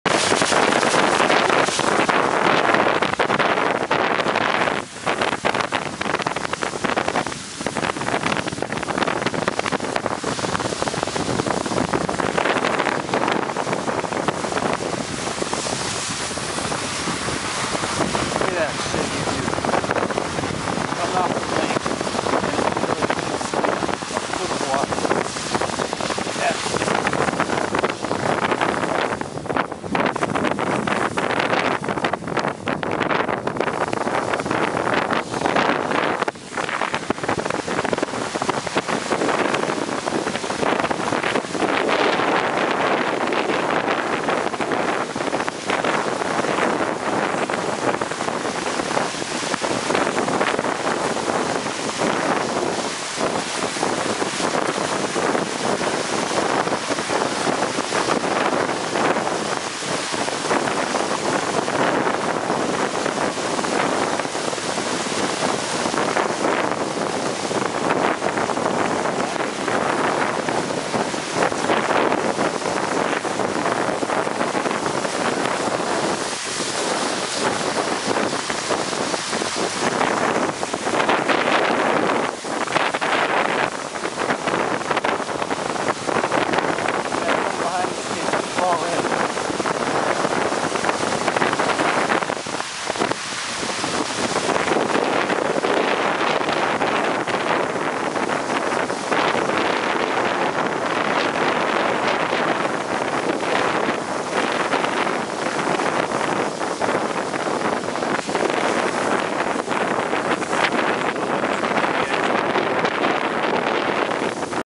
Driving on Water!!!
So we are out on the middle of the lake and we notice there is quiet a bit of water on the lake in the morning, never a good sign because the way back will be worse. To our amazement there is over a foot of water on the lake. Watch this shit!